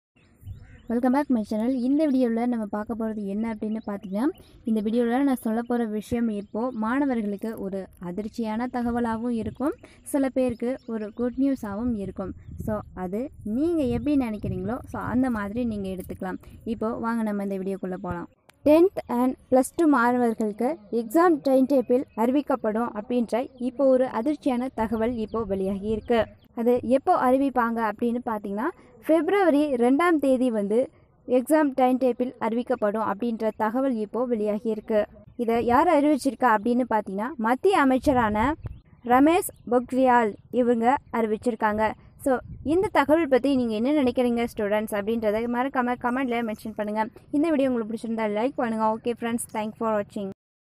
10,12ம் வகுப்பு தேர்வு அட்டவணை பிப் 2ம் தேதி அறிவிக்கப்படும் மத்திய அமைச்சர் ரமேஷ் பொக்ரியால்
சிபிஎஸ்இ 10, 12ம் வகுப்பு தேர்வு அட்டவணை பிப் 2ம் தேதி அறிவிக்கப்படும்: மத்திய அமைச்சர் ரமேஷ் பொக்ரியால் தகவல்..!